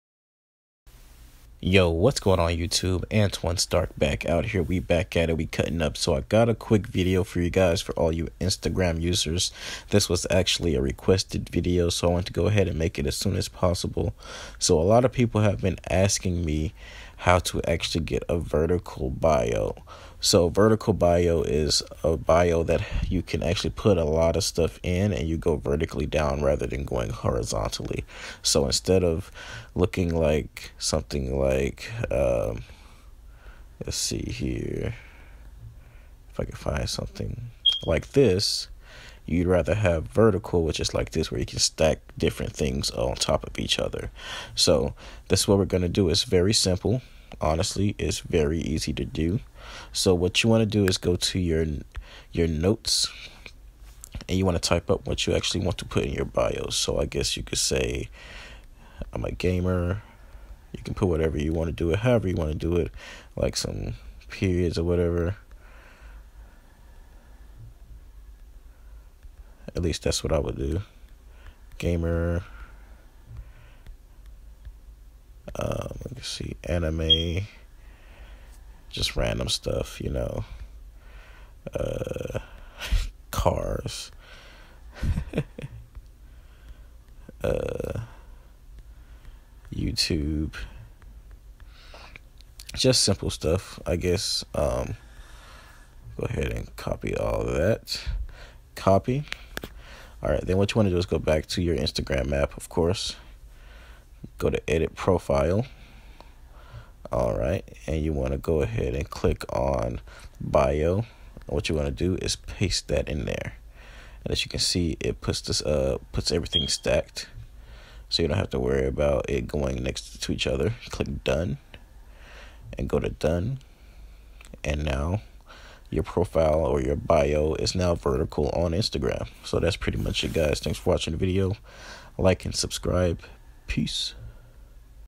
How To Get Stacked Instagram Bio
Hey guys this is just a quick Instagram tutorial, I show you how to get a stacked by you.